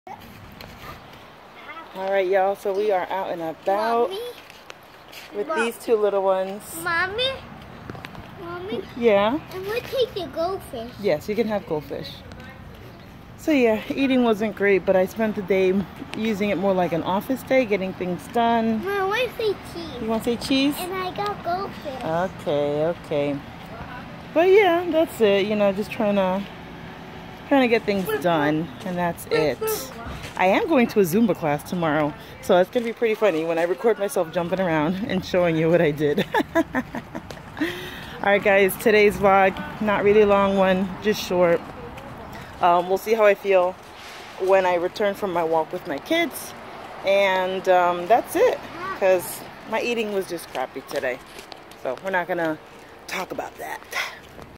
Health Journey 2019 | Ep. 25
Spent the day doing some office work and took my kids to the park. My nutrition hasnt been the best this week.